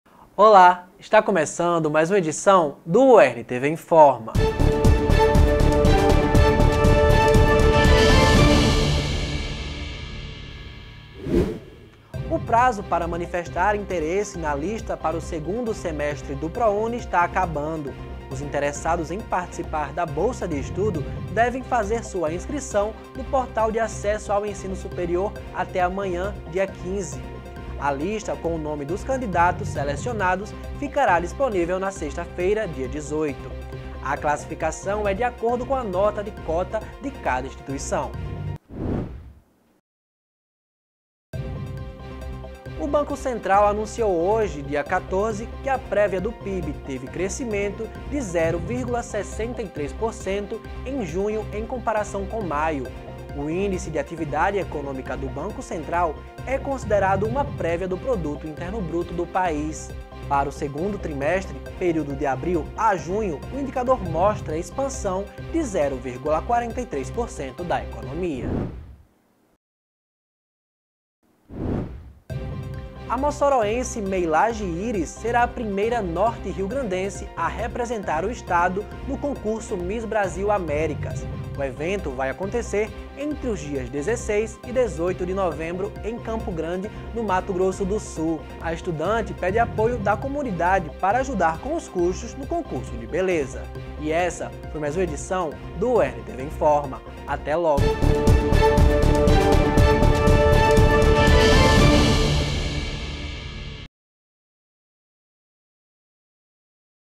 UERN TV INFORMA 14/08/23
Confira as informações do Uern TV Informa de hoje (14/08):

Prazo para inscrições do Sisu acaba amanhã;
Banco central aponta crescimento 0,6% no Pib em junho;
Potiguar pede ajuda para participar do Miss Brasil Américas.

Produção: Rafaela Lúcio
Apresentação: Monalisa Teixeira
Edição: Adson Matheus